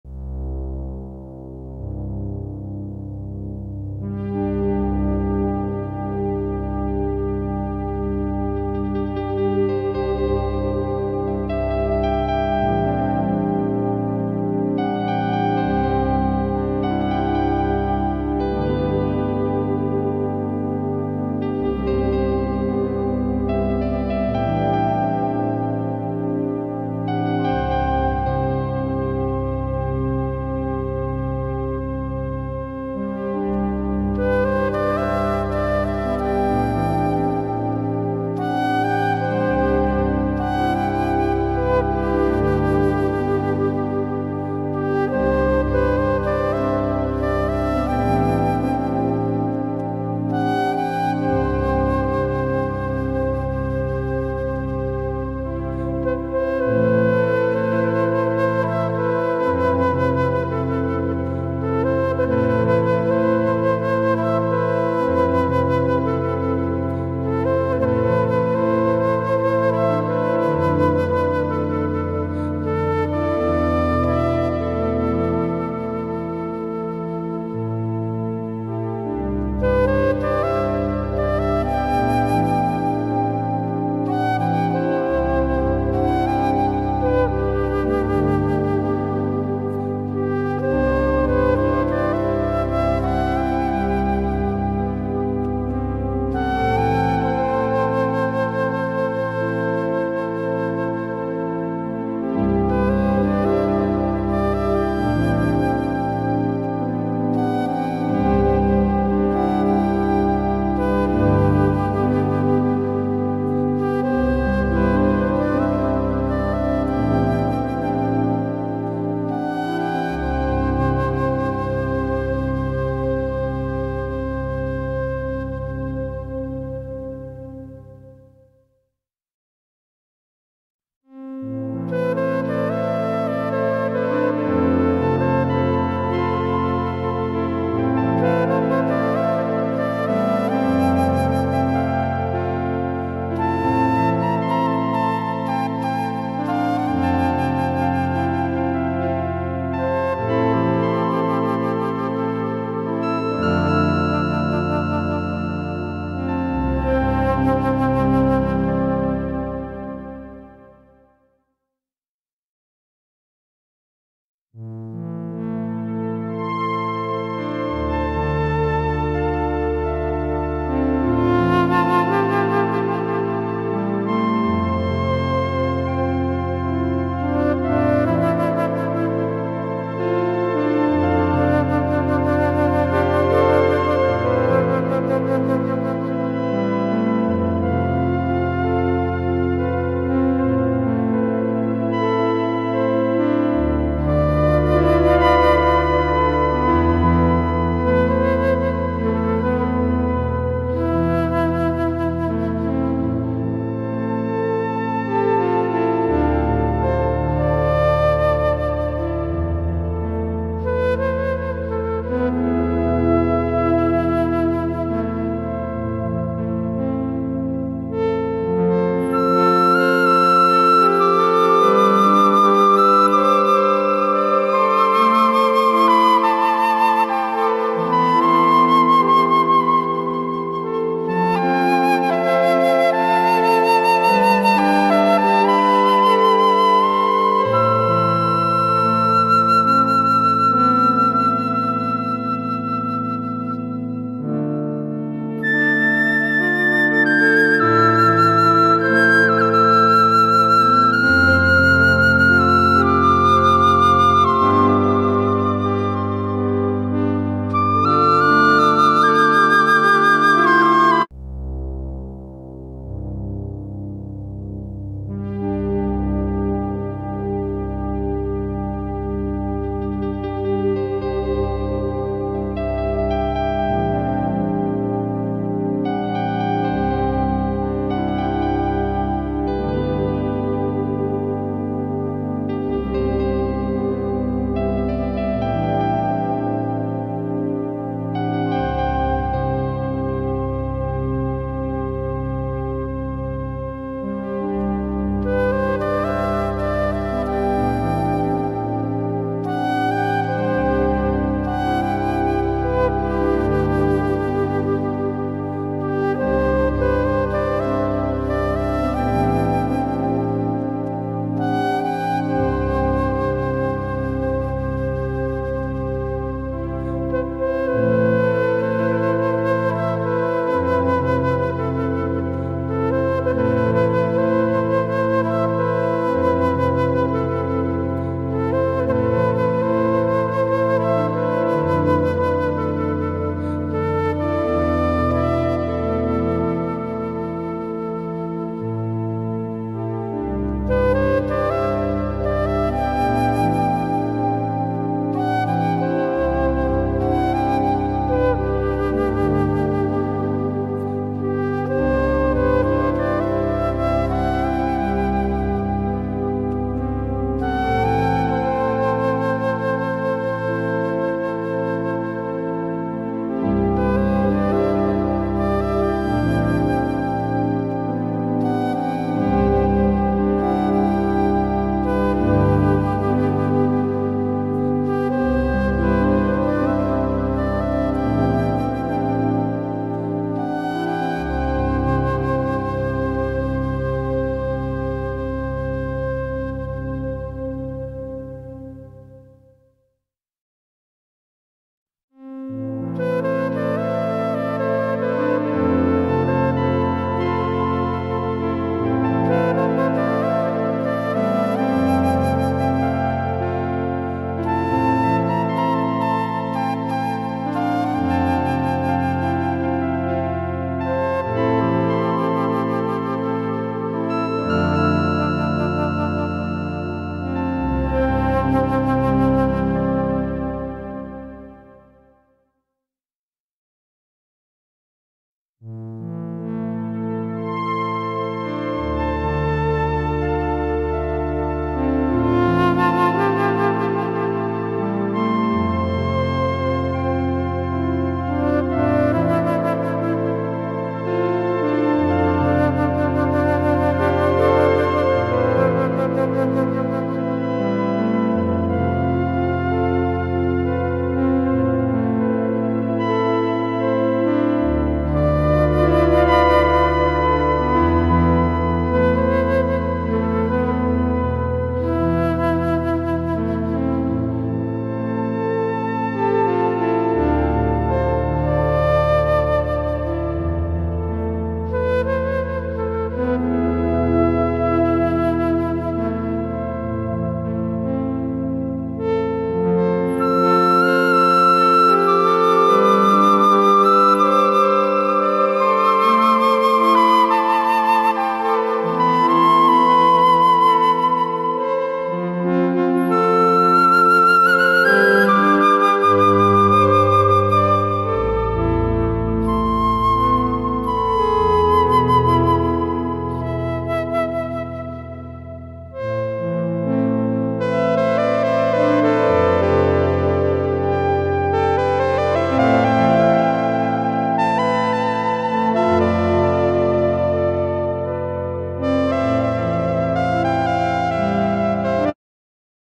432 hZ -sonido natural del universo que calma la mente y promueve la sanación natural del cuerpo *35
Música 432 hZ puede ayudar a mejorar la conexión con la naturaleza, que relaja y calma la mente y el cuerpo o que puede tener efectos curativos